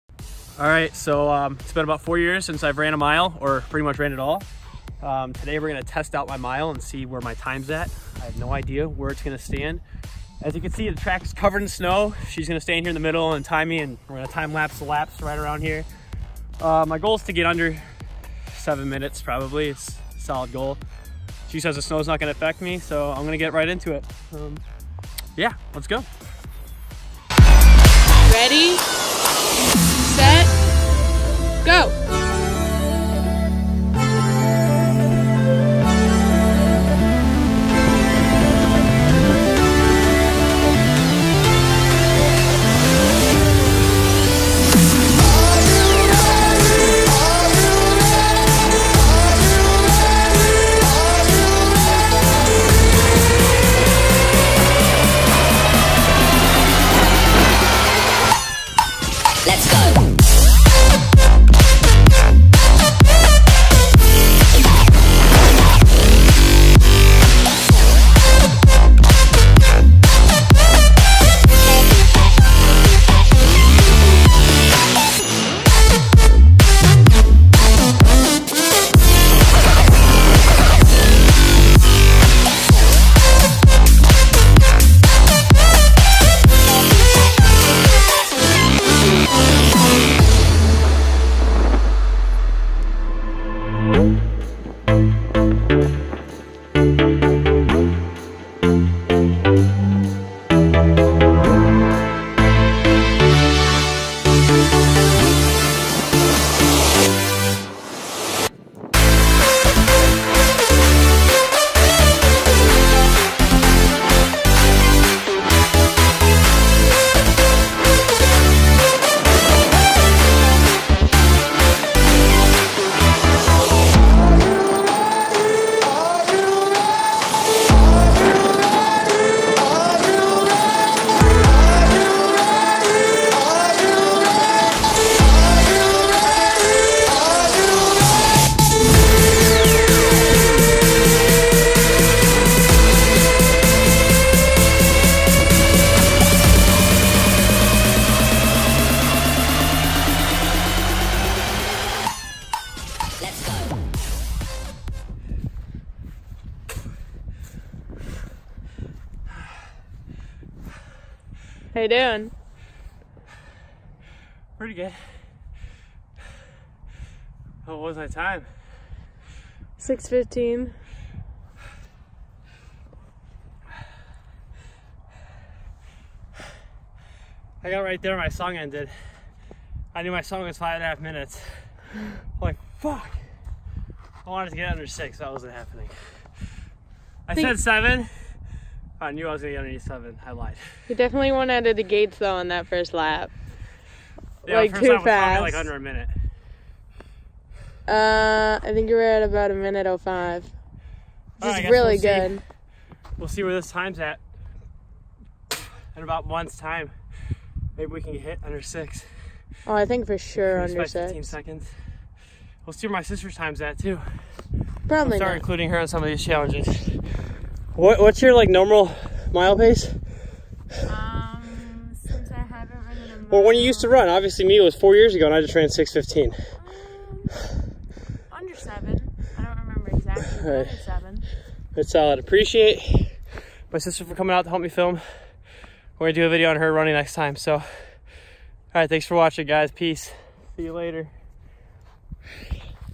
I Ran For The First Time In 4 Years...
this is my first time running in 4  years and I think I definitely didn't do too bad considering I haven't done any cardio at all for months! all I have an updated video in 1 month to see if I can get a sub 6 minute mile!